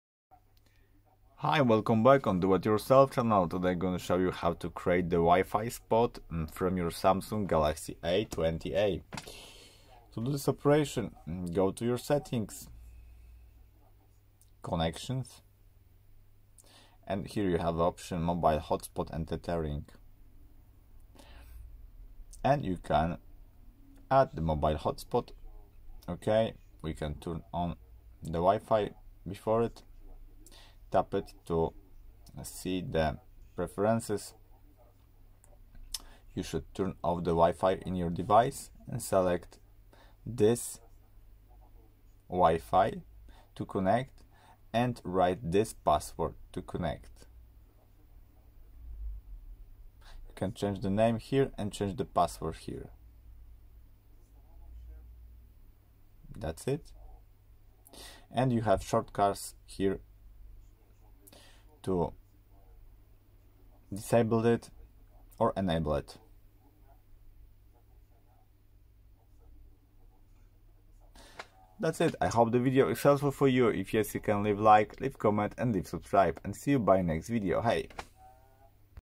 Portable Hotspot SAMSUNG Galaxy A20e - Wi-Fi Sharing
Watch and learn how to enable Portable Hotspot in Samsung Galaxy A20e. Find out all the possible ways of personal hotspot activation. Easily get access to the network on several devices. Share your Wi-Fi Network. Switch it on and off whenever you want to. Personalize your Wi-Fi name and the password.